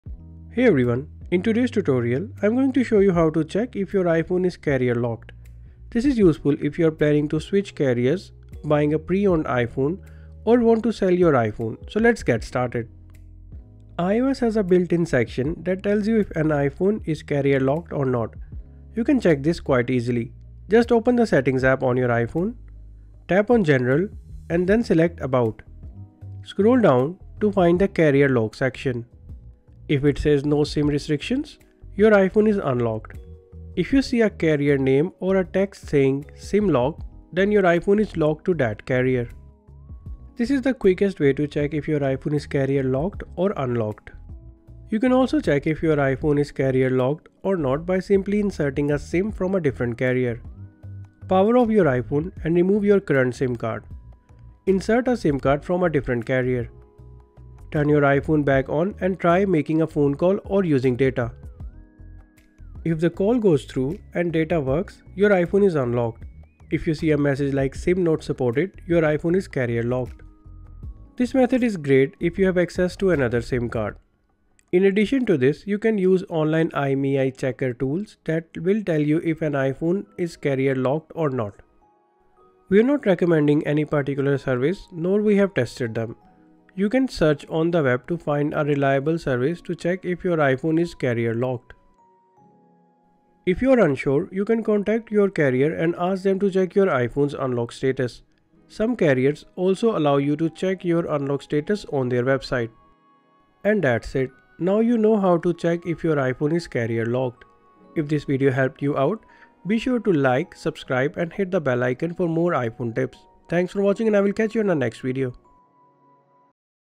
How To Check If iPhone Is Unlocked | Check Carrier Locked
In this tutorial, we show you the steps to check if an iPhone is carrier locked or if an iPhone is unlocked. You can follow these steps to check the status of the carrier lock on your iPhone 16, iPhone 15, iPhone 14, and older models.